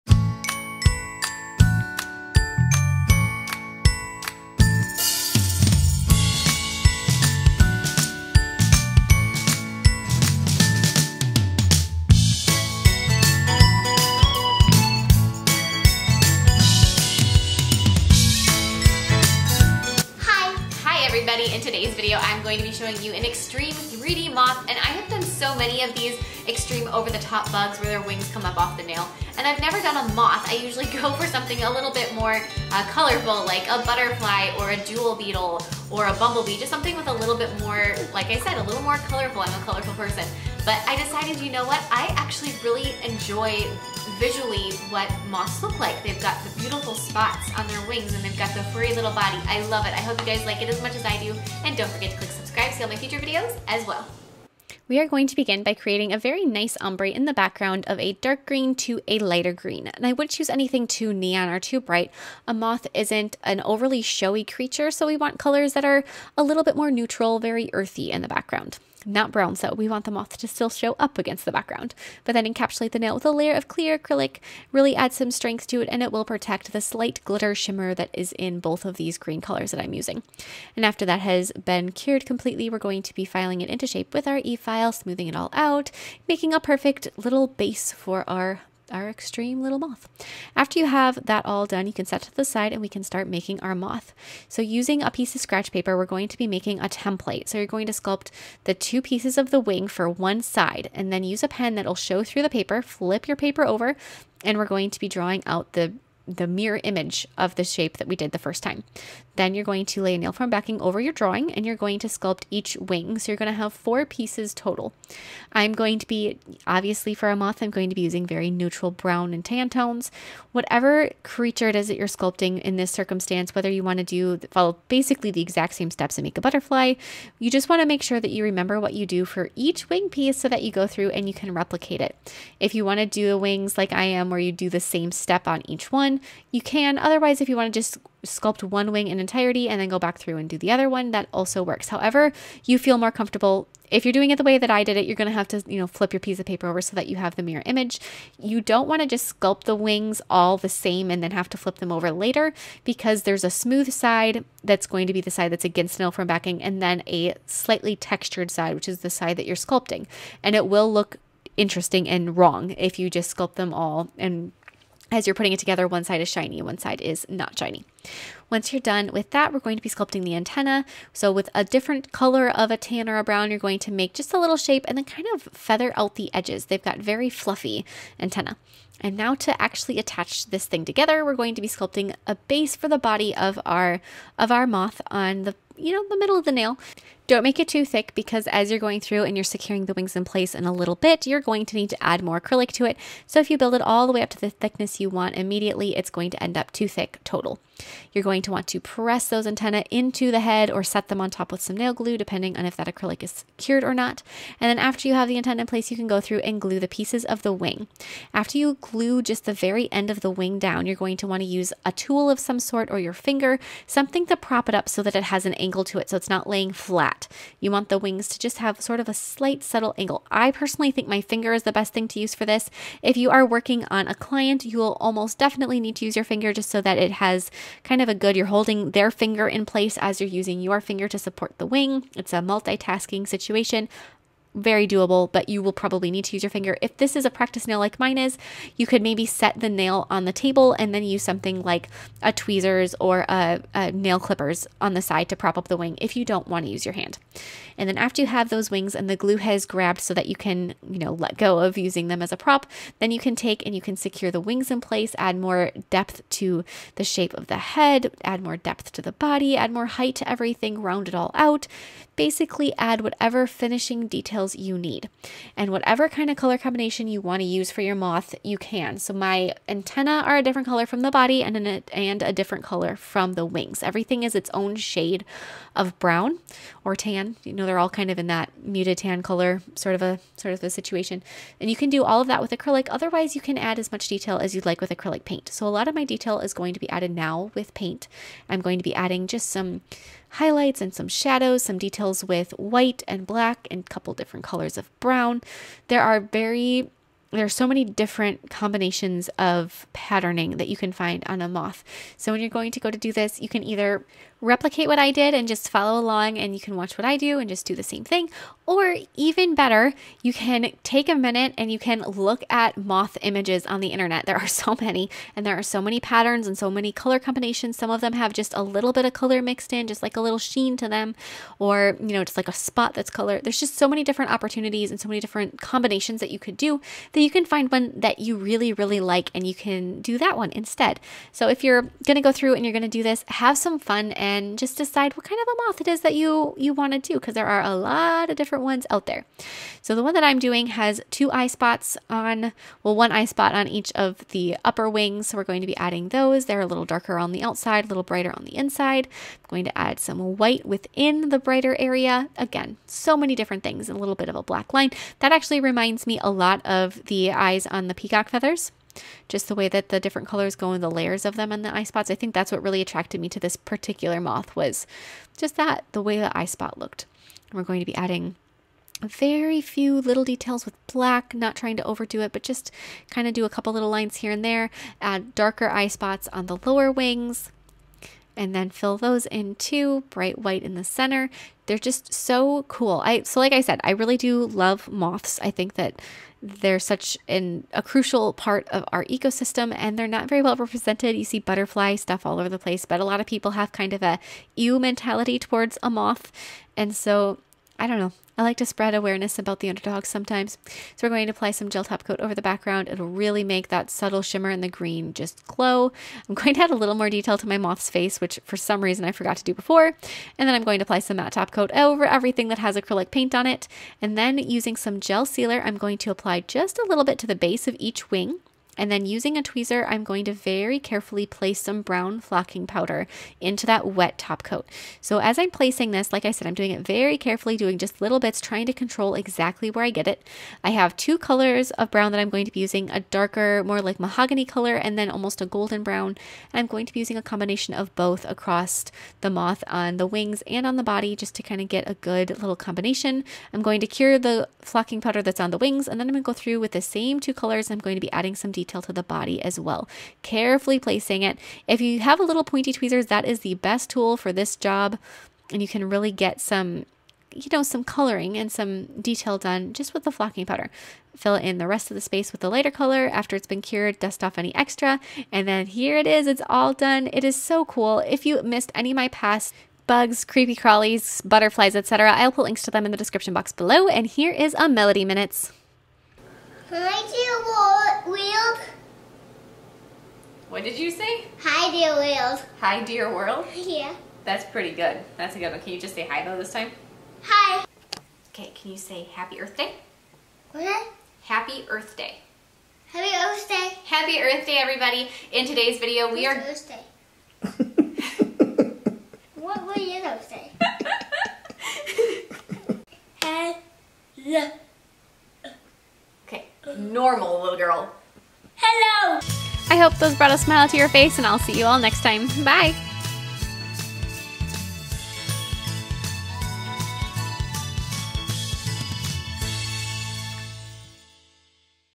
EXTREME 3D Moth Acrylic Nail Art Tutorial
Who else just love seeing fluttering moths and butterflies in their garden?

Double Dip Colors Used In This Video ~ Green elixir 823, woolen mittens 786, sonata rose 561, toffee pecan 469, espresso mauve 606